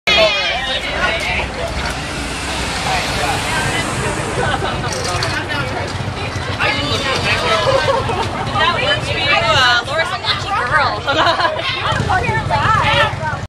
Zombie Pub Crawl 2010 - VID00143.mp4
- Minneapolis Zombie Pub Crawl 2010 - Taken in front of Maxwell's on Washington Ave S in the Mill District of Minneapolis. ZPC6